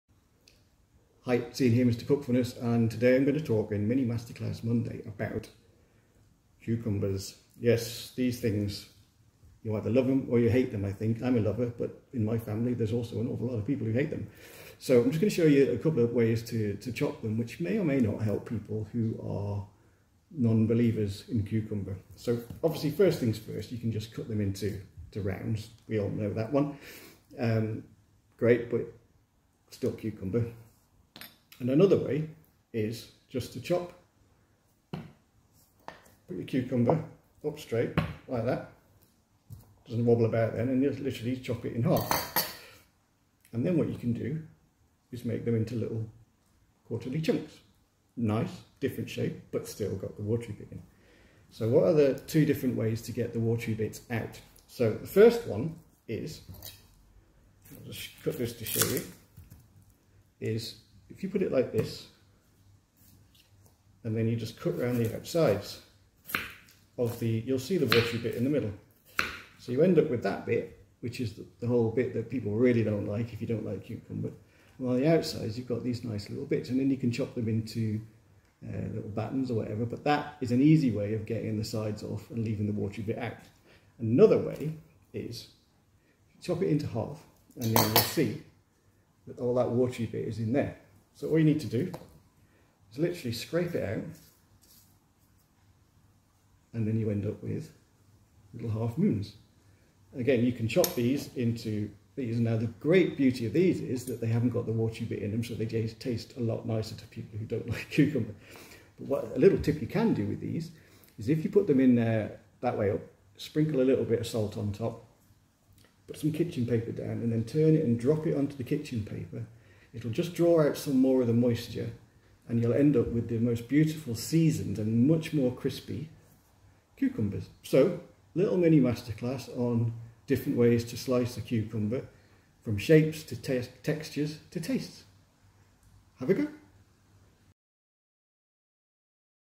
Cucumbers, a real love/hate! Here's a couple of quick ways to chop & prepare for a real difference!
Mr Cookfulness, Ian Taverner, shows you ways to chop and prepare cucumbers to get rid of the watery nastiness inside and maybe just convert the cucumber non believers!